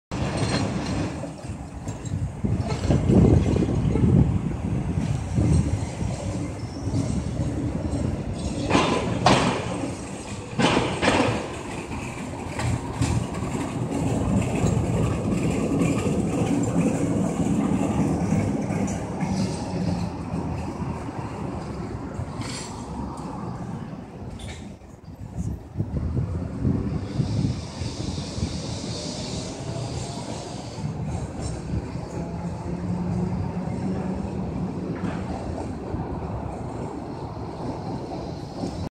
71-619К 5124 На Нижней Красносельской улице
Трамвай был построен 12.2004

ОПИСАНИЕ

ОПИСАНИЕ ПРО 71-619К 5124

Поступил Трамвайное депо им. Баумана
Передан Октябрьское трамввйное депо
Работал на маршруте №20
Заводская табличка отсутствует
Передан Трамвайное депо им. Русакова